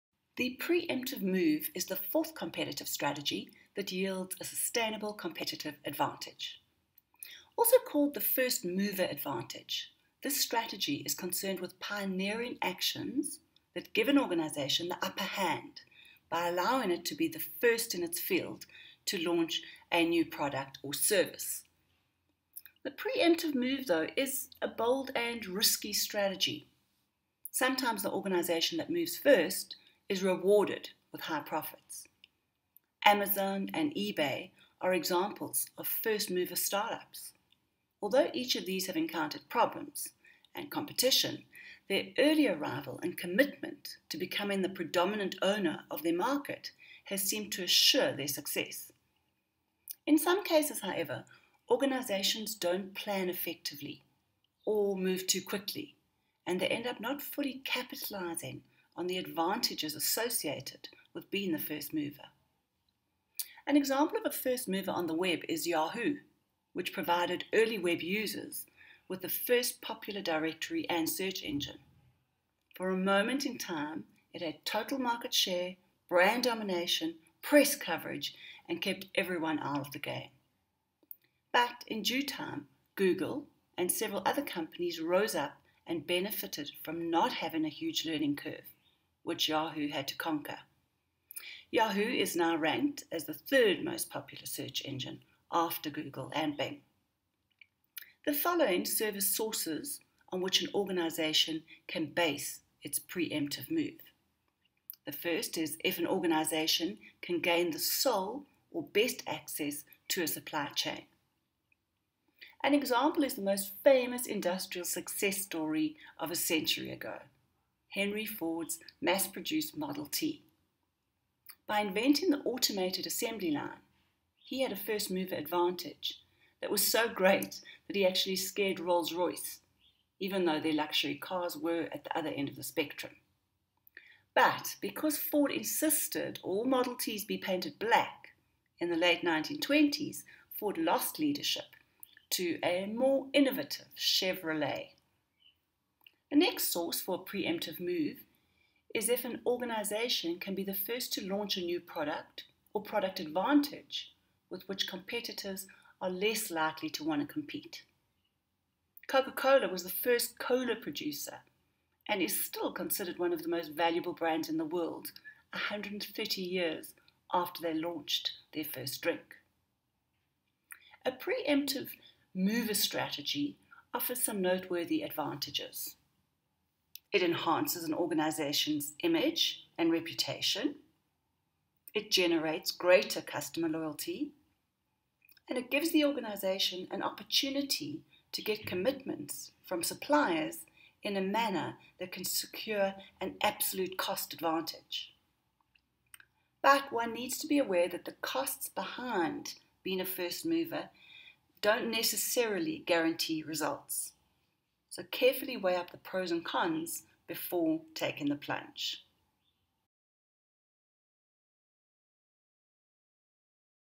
COMPETITIVE STRATEGIES - PART FOUR "THE PRE-EMPTIVE MOVE"
An in-depth explanation of the 'pre-emptive move' in competitive marketing strategies.